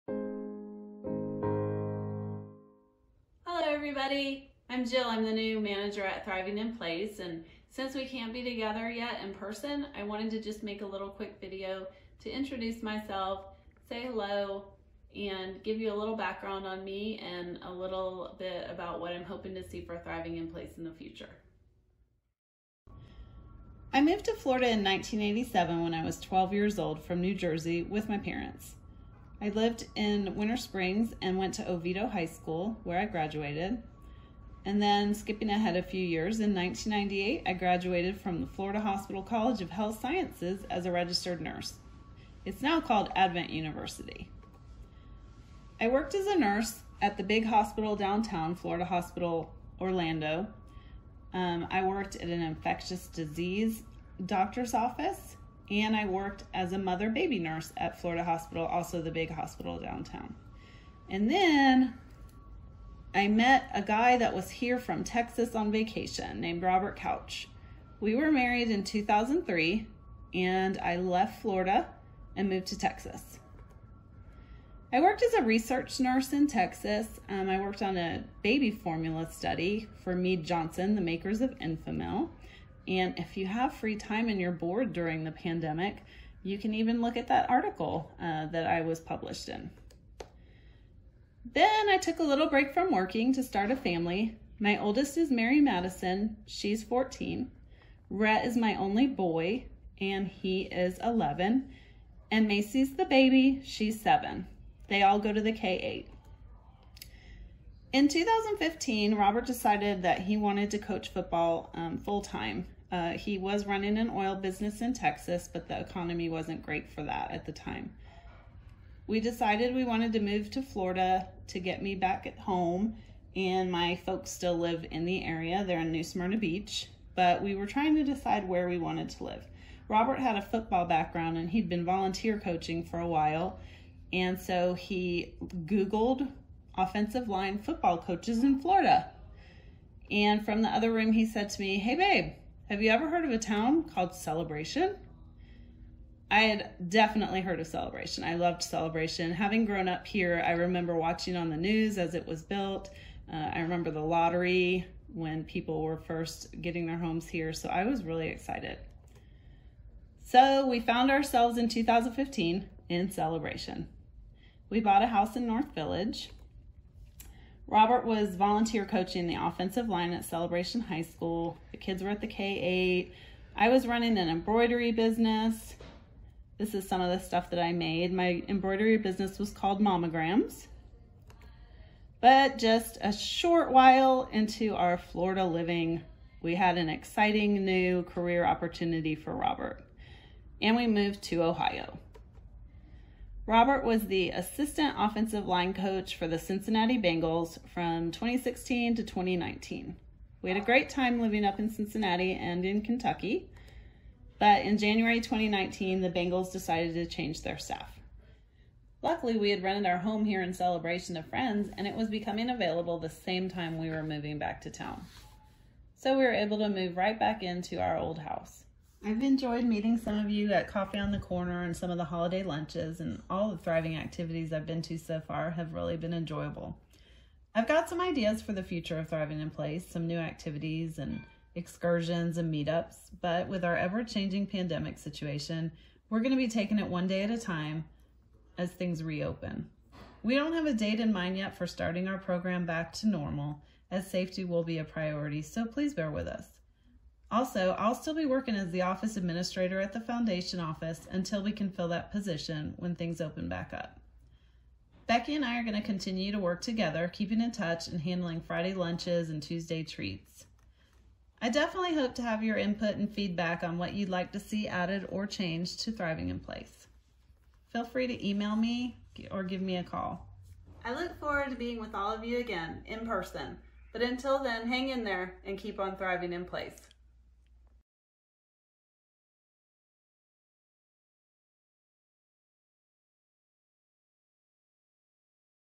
Meet Jill Couch, The New Thriving In Place Manager
A little bit about Jill Couch, the new Thriving In Place Manager.